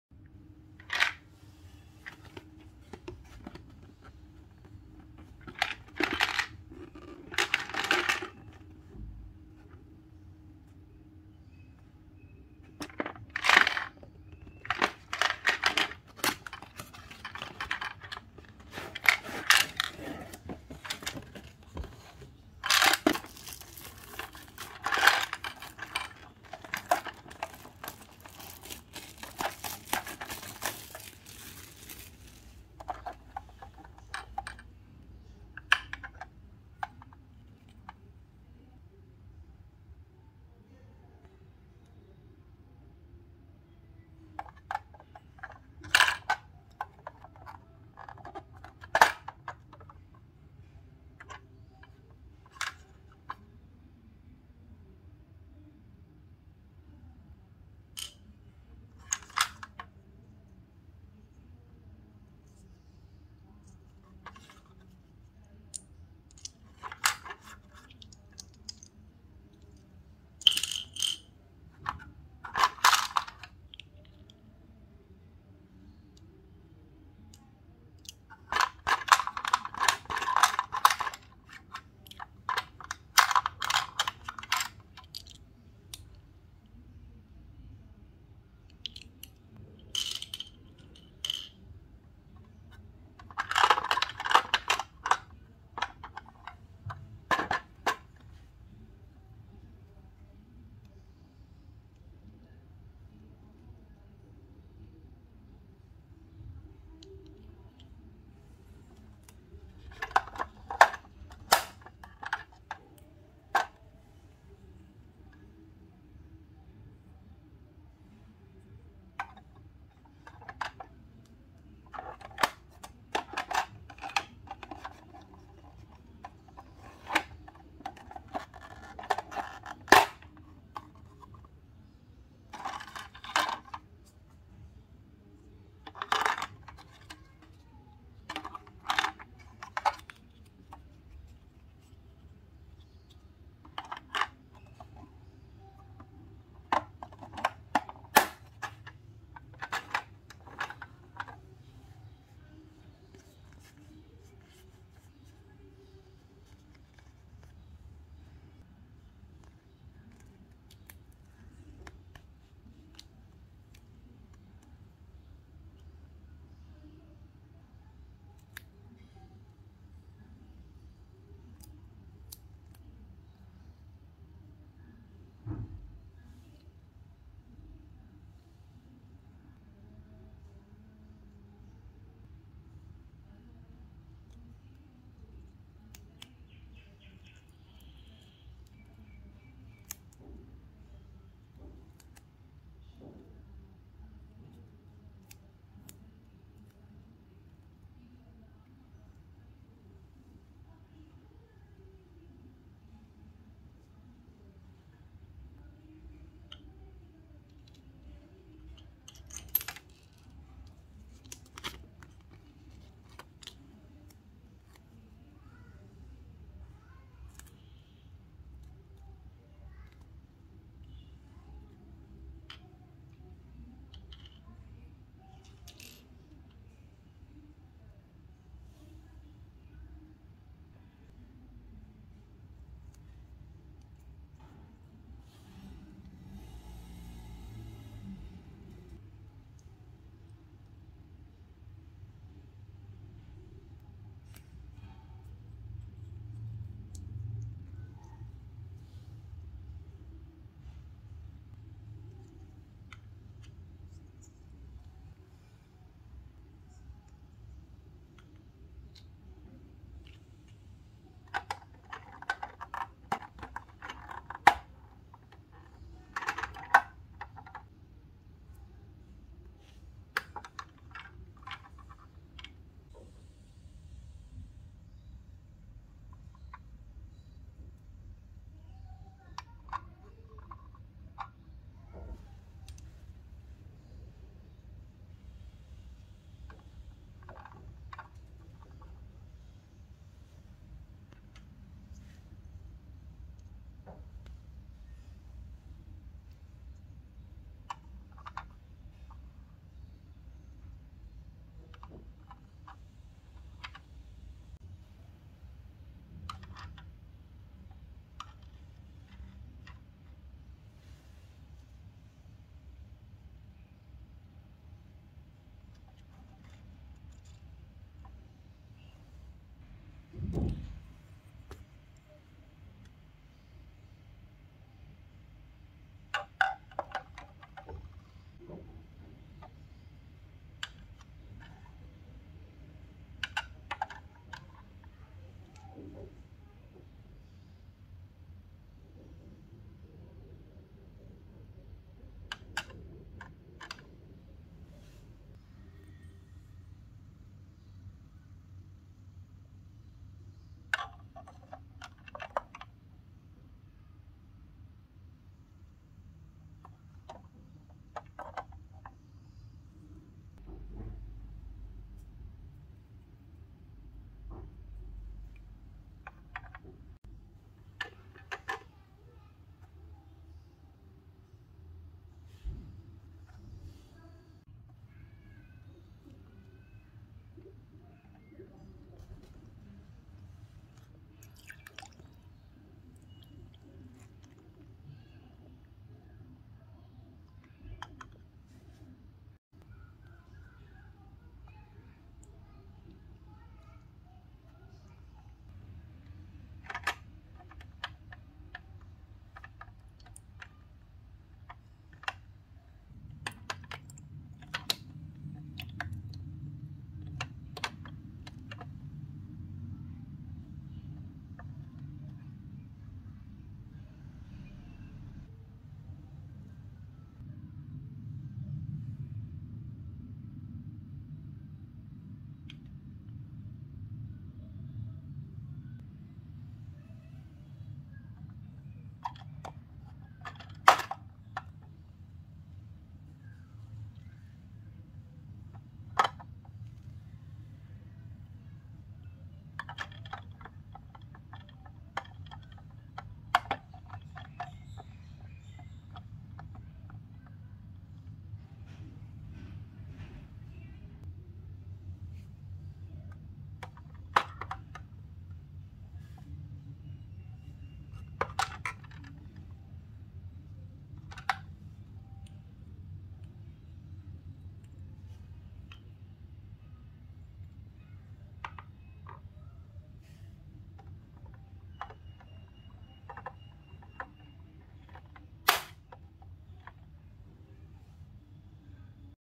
asmr Satisfying unboxing toy dollhouse refrigerator with real working water dispenser 💦
asmr Satisfying unboxing toy dollhouse refrigerator with real working water dispenser 💦 miniature no Music